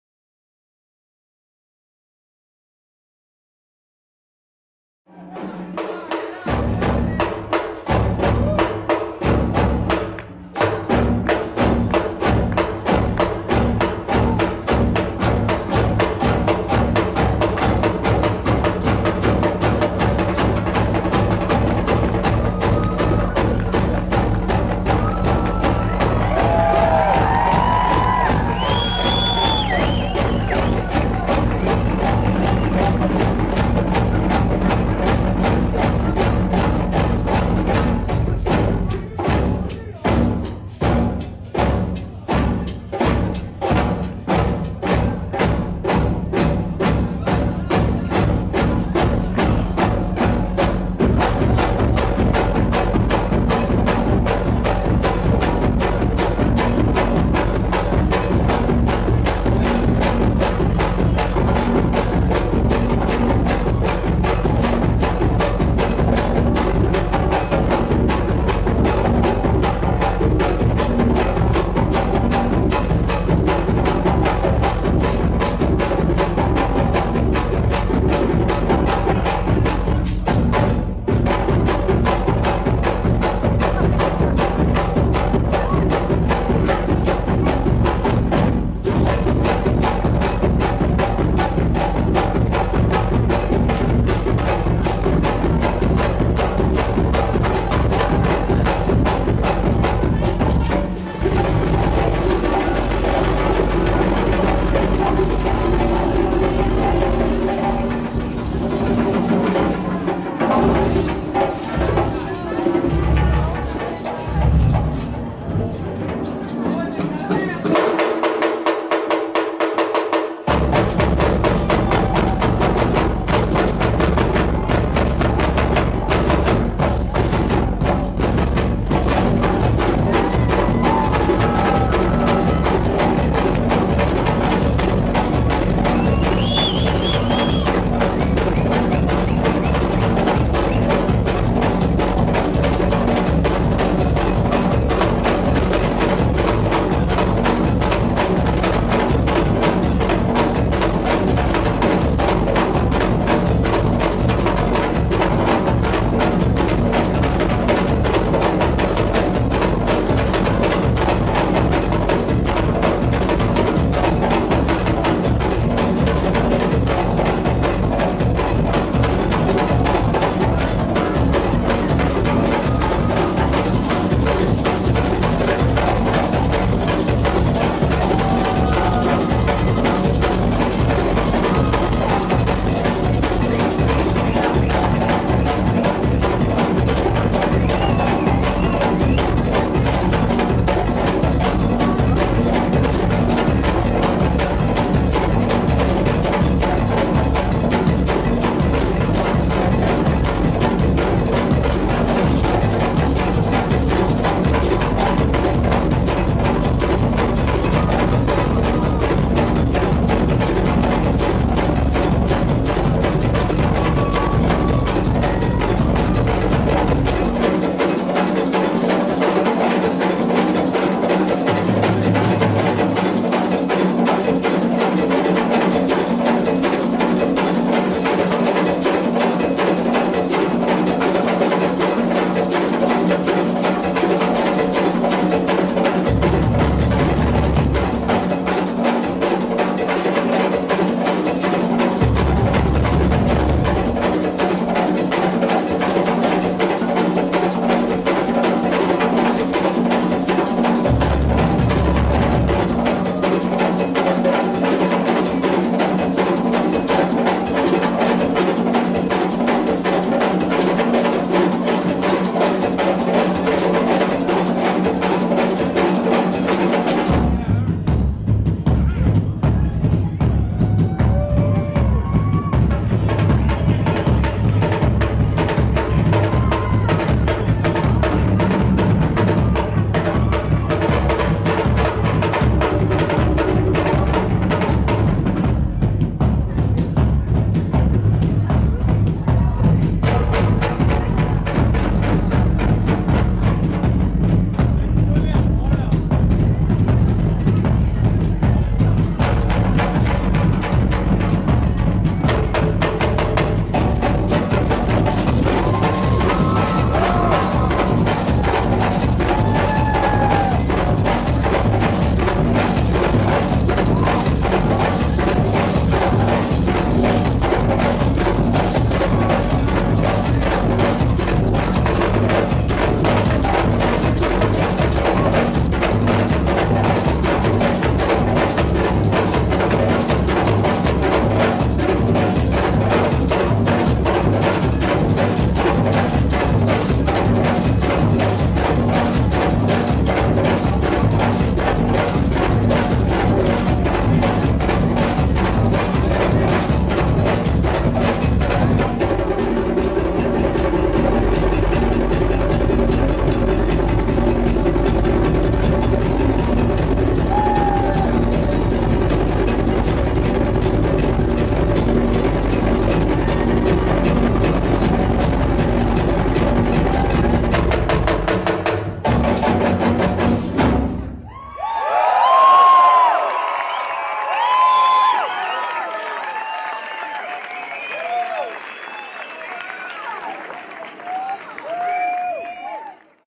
La Bomba del Tiempo!!!!! (Buenos Aires)
The Live show of La Bomba del Tiempo In Buenos Aires... What a Vibe!!!